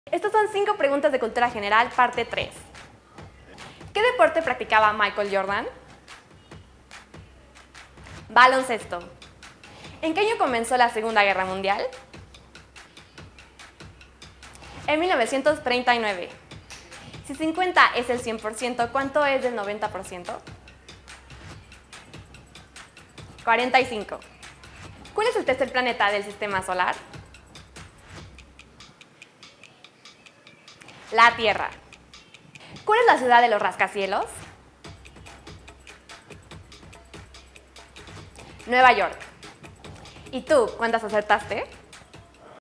🤔📒📗🤔5 PREGUNTAS DE CULTURA GENERAL!!!!🌎📚📕🌏 - PARTE 3
🤔📒📗🤔5 PREGUNTAS DE CULTURA GENERAL!🌎📚📕🌏 ¿CUÁNTAS ACERTASTE?
PARTE 3
Marlenne Gonzáles te trae 5 preguntas básicas de cultura general!. ¿lograste responderlas? 🧐🧐🧐👩‍💻👩‍💻👩‍💻